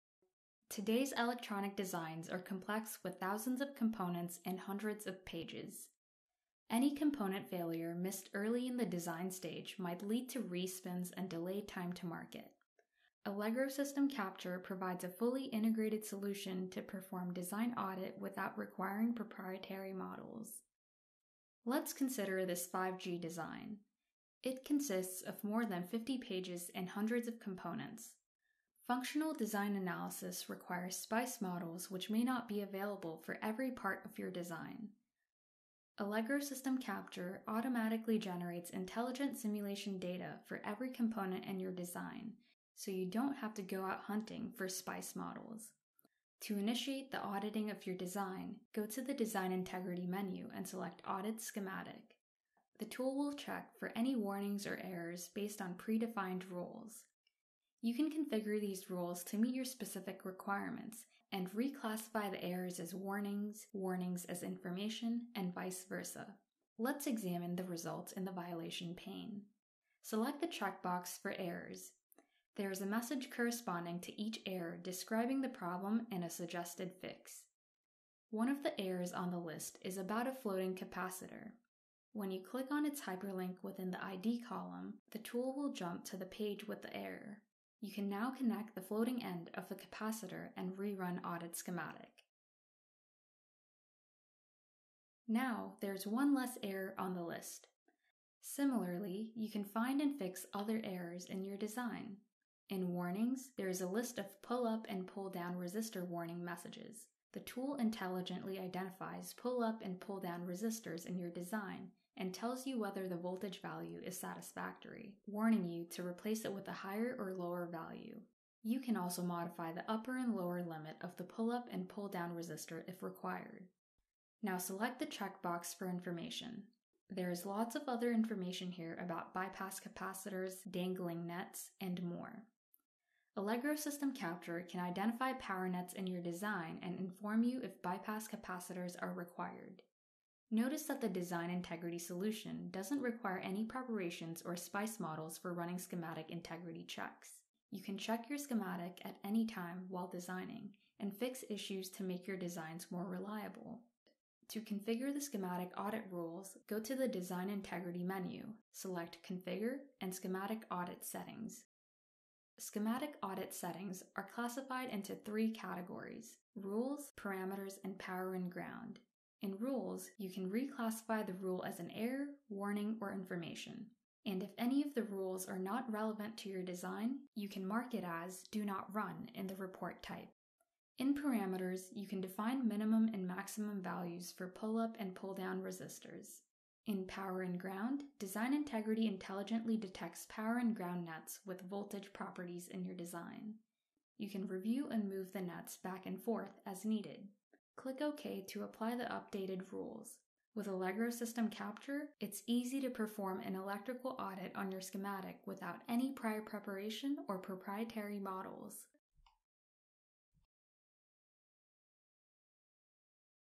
Ensure design reliability using schematic integrity checks | Allegro System Capture
Today’s electronic designs are complex with thousands of components and hundreds of pages. Any component failure missed early in the design stage might lead to re-spins and delay time to market. Allegro System Capture provides a fully integrated solution to perform design audit without requiring proprietary models.